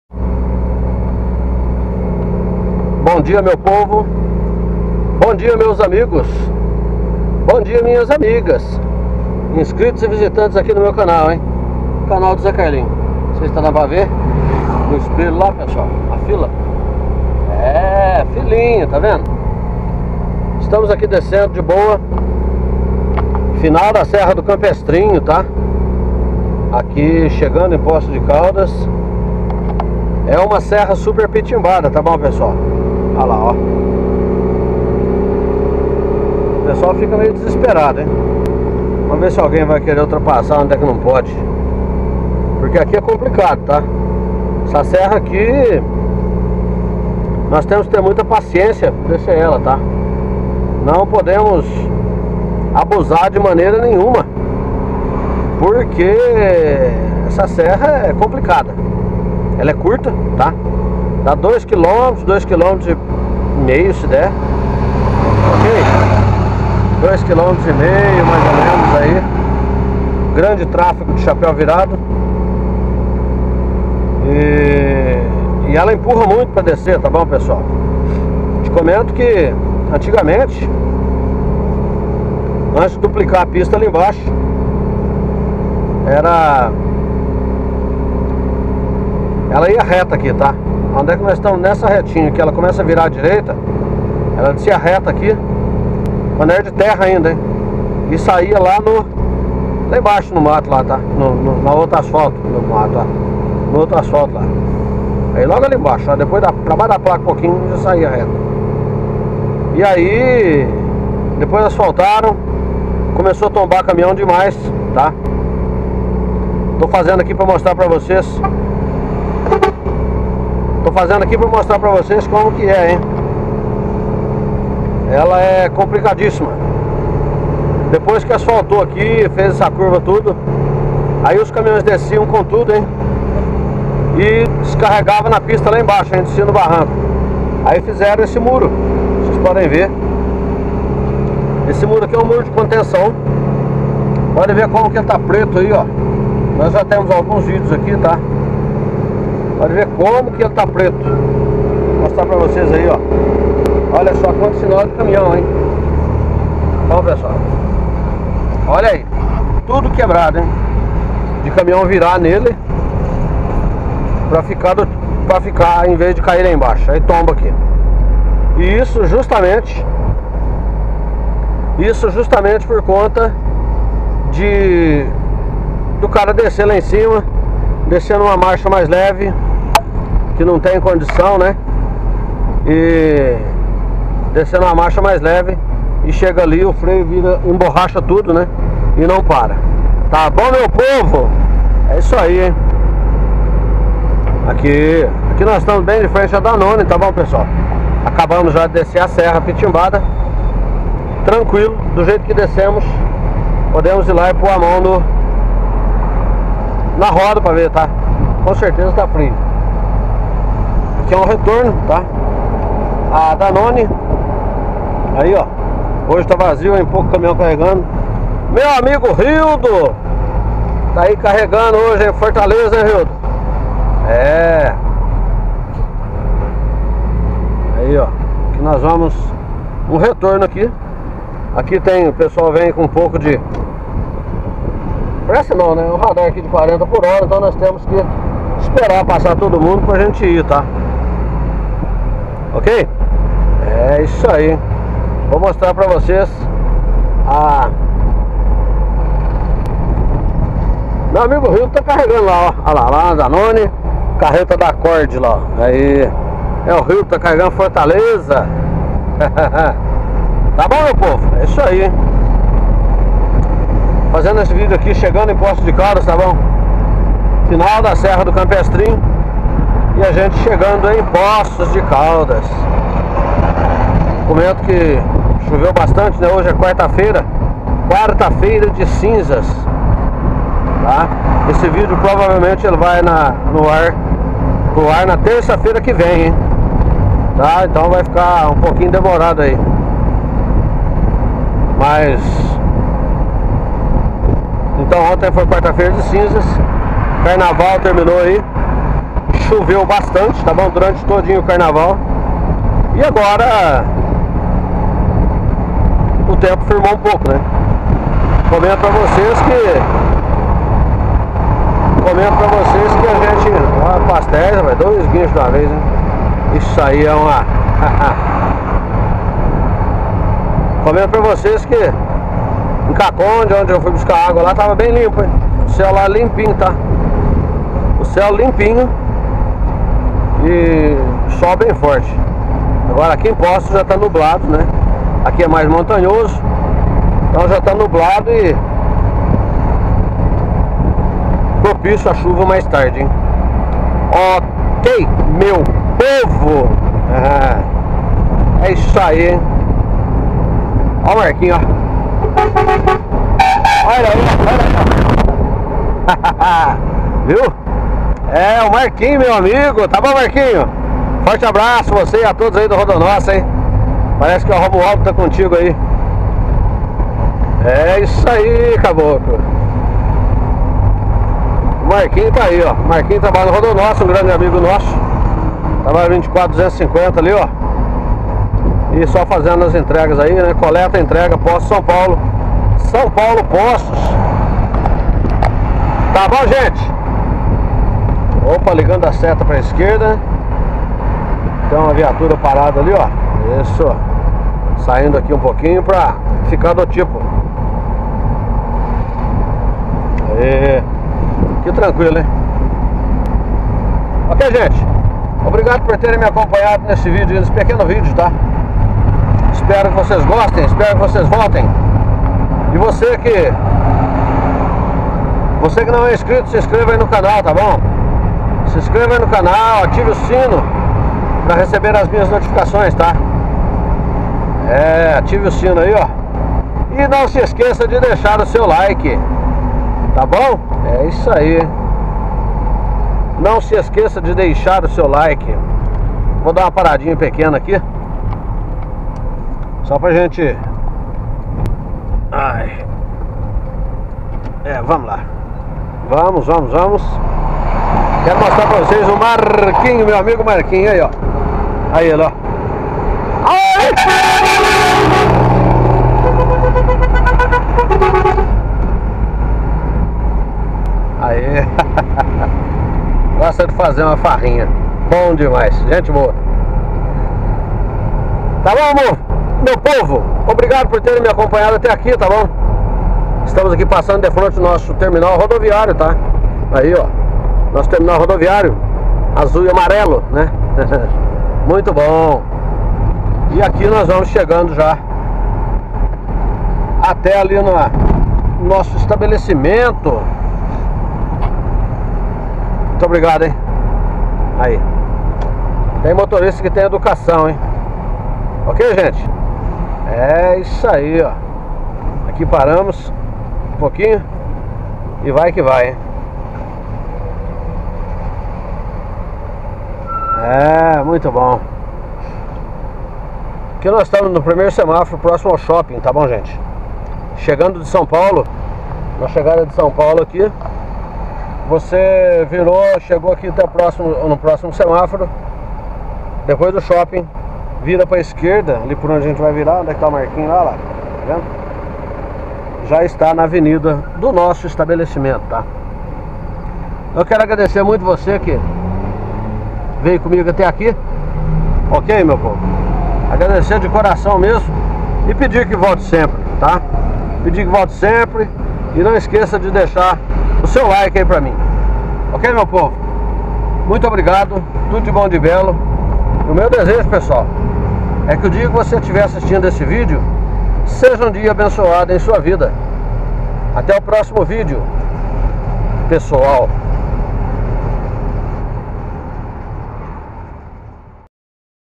(079/19) VW 8150 FINAL DA PERIGOSA SERRA DO CAMPESTRINHO E A CHEGADA NA CIDADE DAS ROSAS
VW 8150 FINAL DA PERIGOSA SERRA DO CAMPESTRINHO E A CHEGADA NA CIDADE DAS ROSAS
Compartilhando um pouquinho e beleza de perigo em meio a bela natureza.